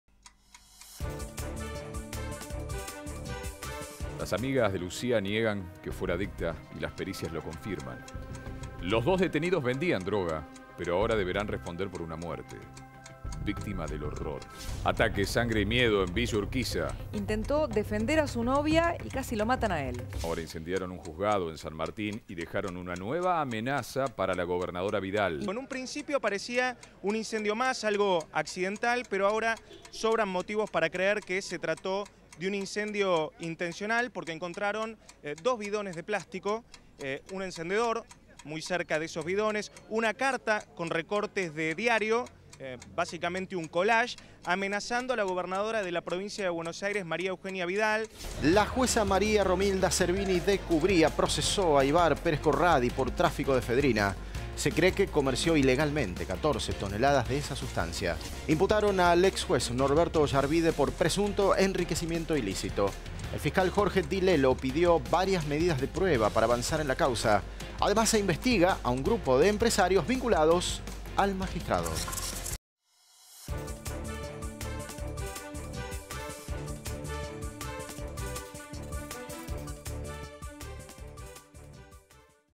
Minuto RSS - Telefe Noticias
En un minuto te contamos las noticias más destacadas del día. Crimen de Lucía; ataque, sangre y miedo en Villa Urquiza; incendiaron un juzgado en San Martín; procesaron a Pérez Corradi; el exjuez Oyarbide fue imputado. Seguinos de lunes a viernes a las 21:30 por nuestro canal en YouTube. Informate con Minuto RSS.